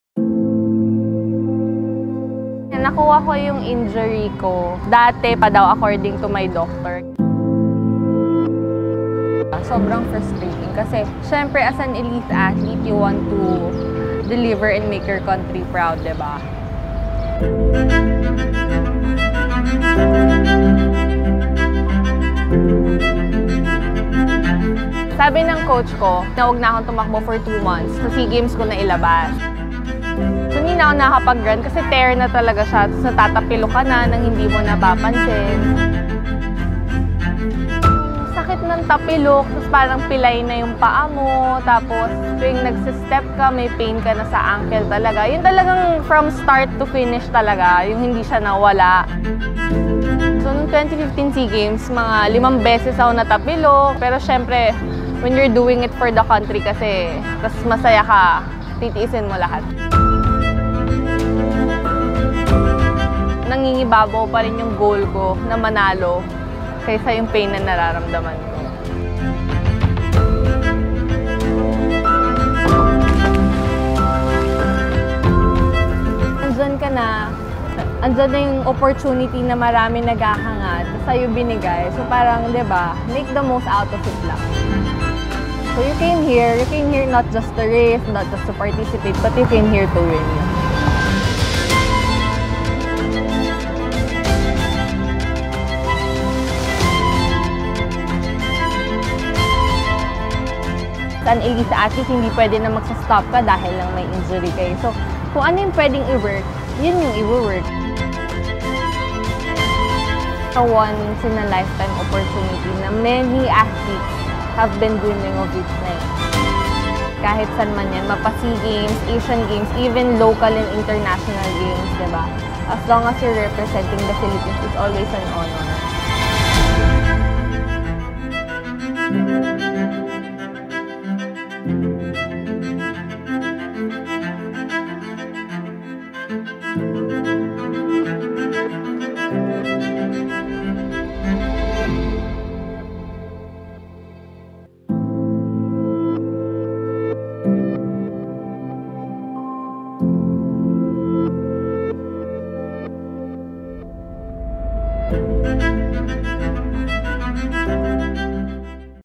Claire Adorna: SEA Games Gold Medalist
Powering through pain for the love of her sport and country, Claire Adorna overcomes the challenges of an old injury. Watch how Claire carried her country's pride as she shares her SEA Games journey.